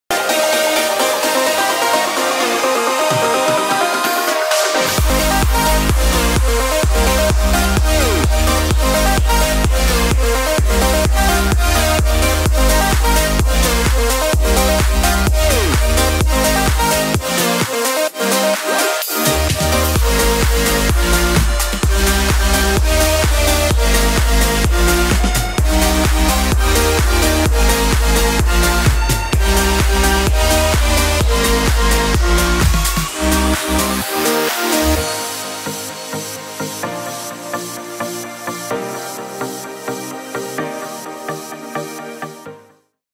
TELESIN Vlog Selfie Stick with Bluetooth Remote Control gopro smartphones remote control
❤️TELESIN Vlog Selfie Stick with Bluetooth Remote Control. The remote control is not only for GoPro10/9/8/Max but also compatible with the smart phones.
✅Super compact, the whole set weighs only 110g.
✅The remote control range is up to 72 m